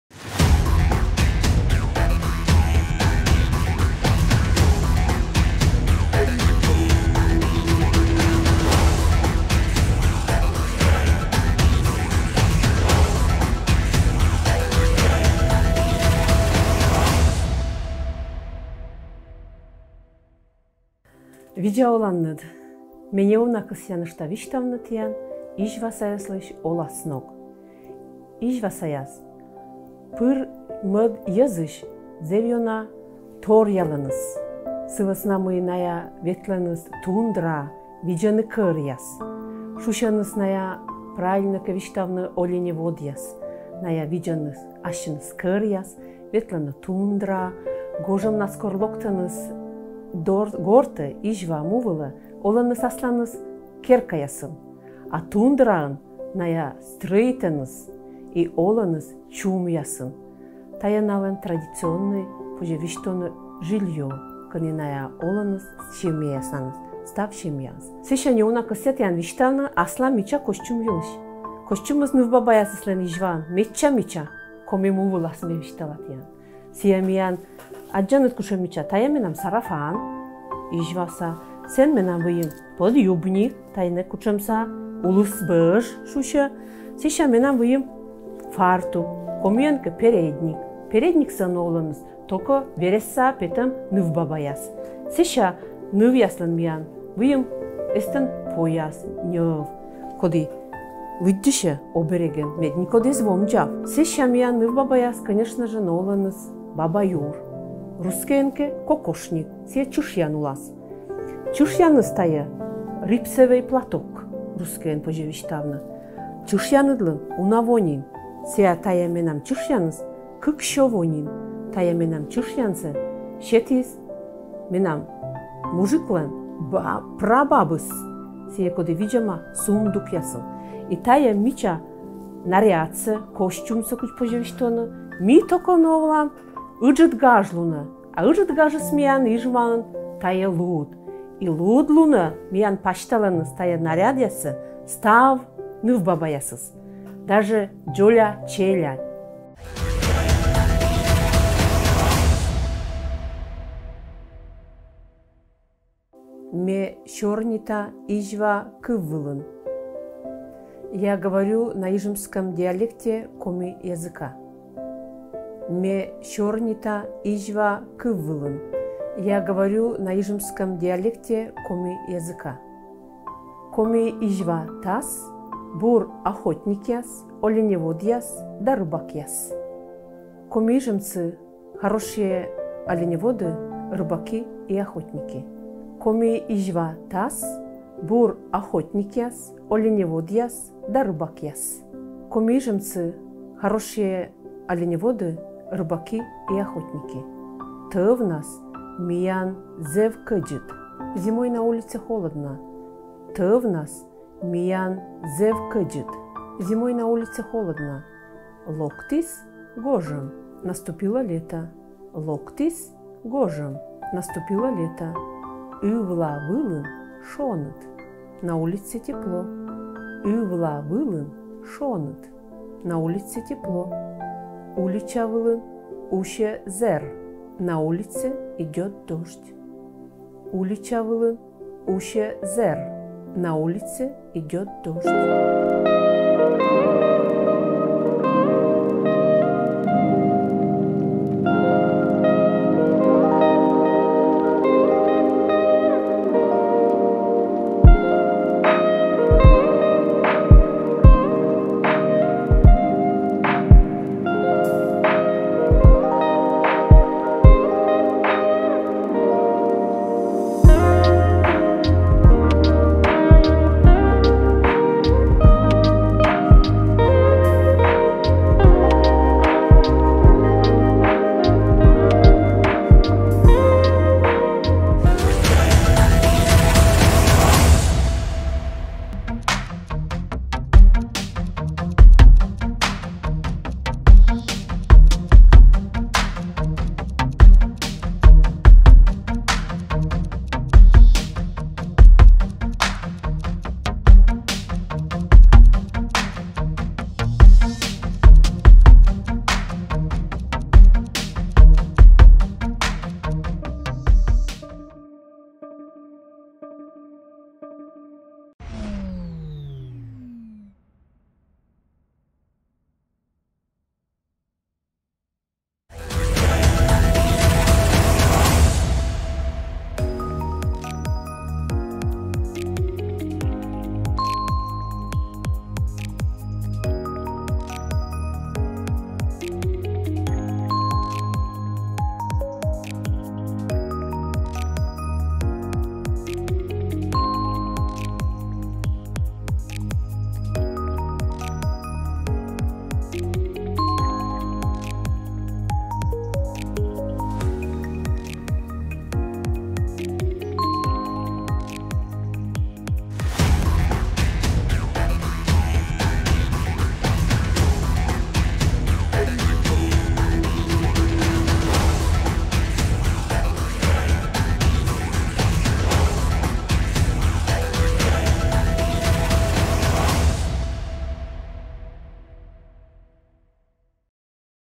Коми язык | Тема 3 | Быт
Третья серия языковых курсов посвящена быту коми-ижемцев. Галина Григорьевна Ануфриева рассказывает о жизни и о традиционном костюме своего народа. Интерактивный урок в конце ролика поможет вам запомнить несколько полезных выражений и закрепить произношение.